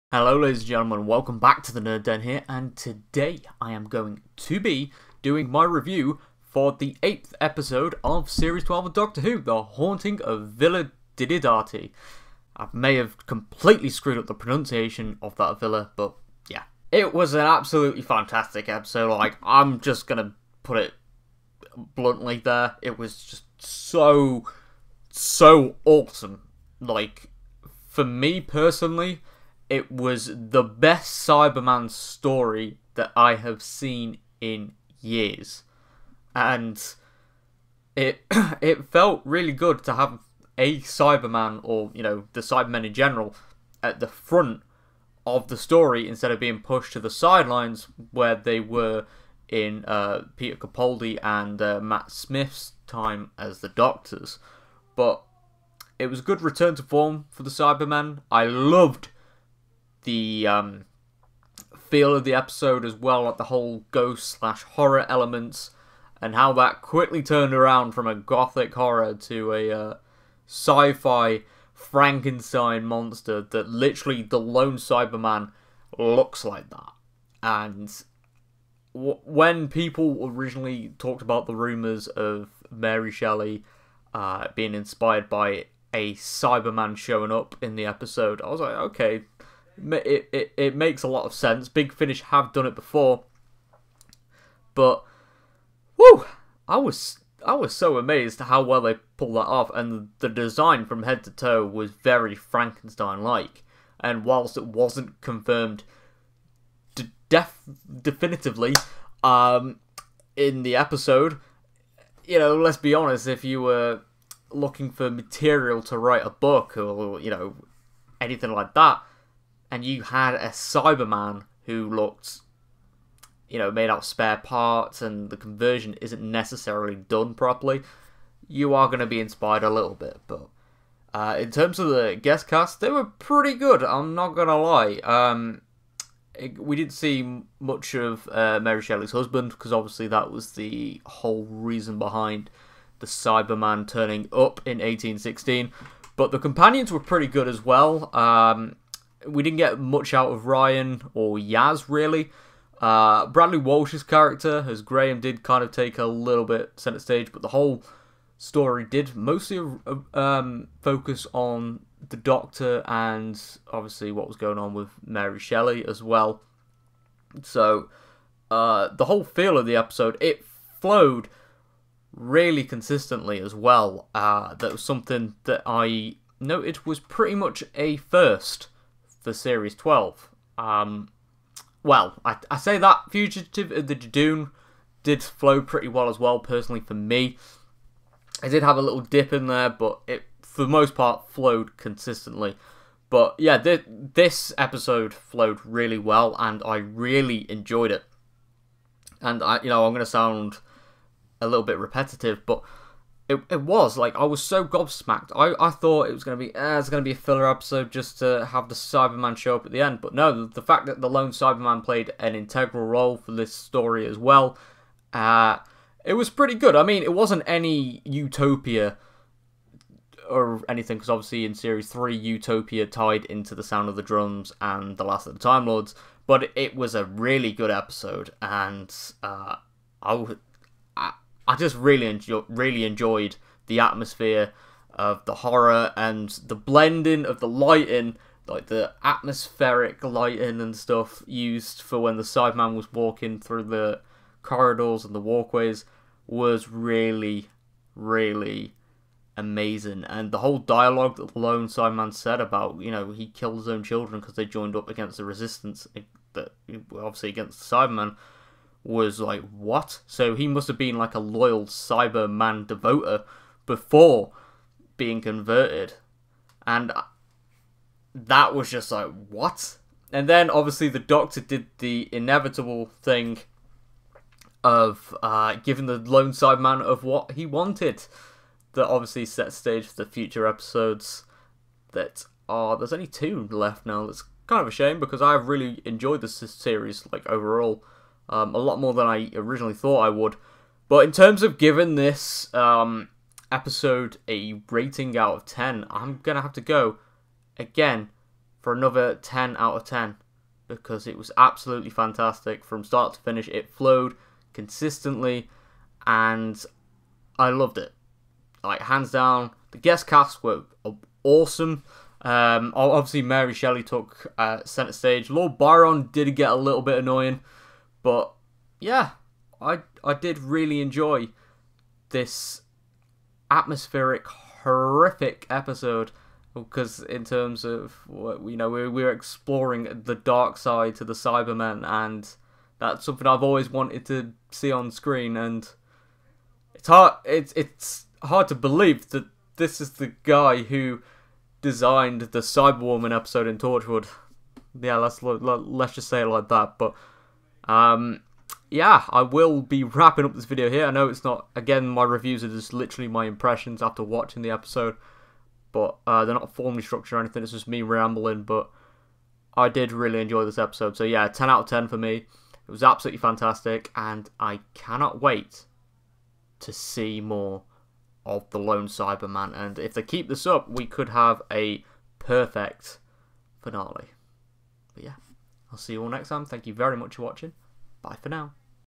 Doctor Who "The Haunting Of Villa Diodati" Review! (Doctor Who Series 12 Episode 8 Review)
In this video, I will be reviewing the 18th episode of series 12 of Doctor Who, which is titled "The Haunting Of Villa Diodati"! :D